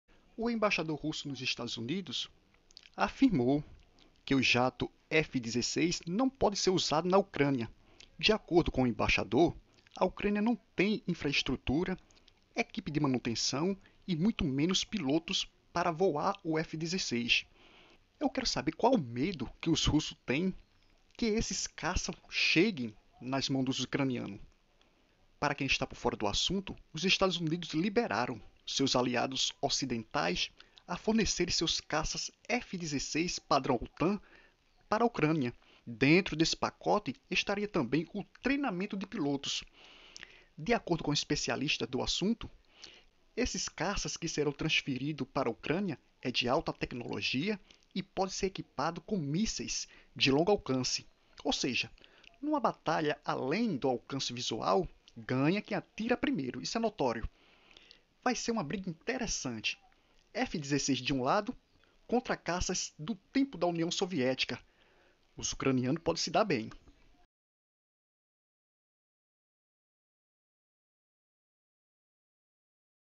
Embaixador russo diz que jatos F-16 não podem ser usados na Ucrânia
O Embaixador russo Anatoly Antonov afirmou no domingo que jatos F-16 não podem ser usados na Ucrânia.